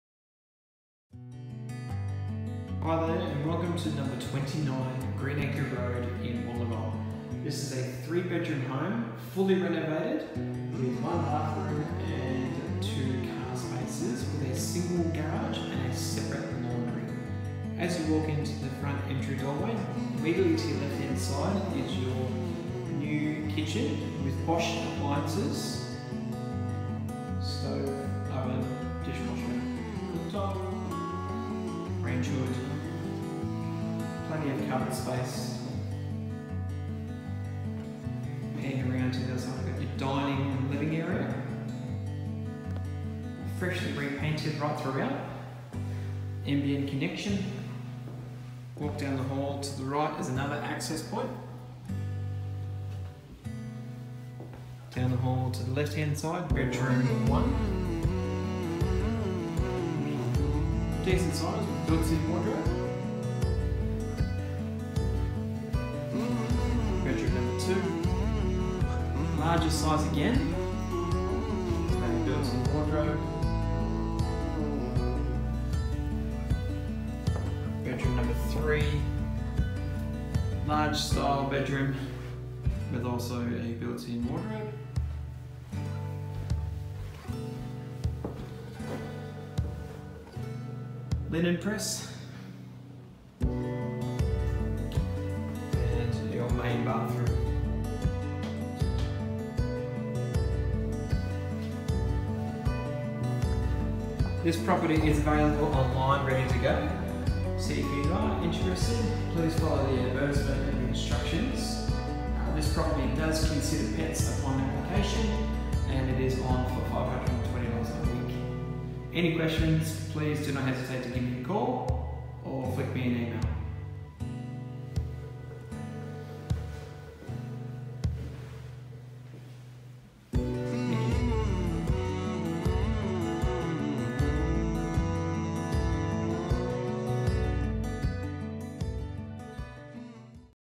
Online Rental Viewing - 29 Greenacre Road, Wollongong
Take a tour of this property currently available for lease with MMJ Real Estate. For more information or to apply online head